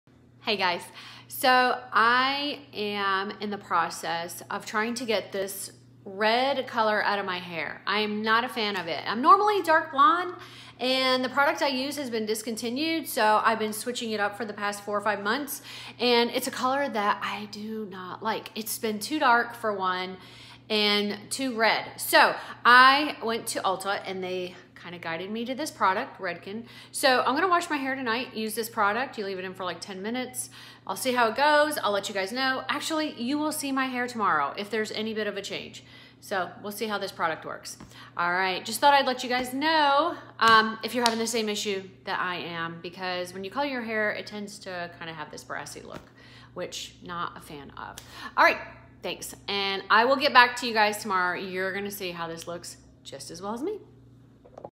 BEFORE USING REDKEN BLONDAGE EXPRESS ANTI BRASS MASK
This video shows what my hair looked like before using the Redken mask to help "calm down" the orange tones that have gotten way out of hand.  Be sure and visit the second video to see the results.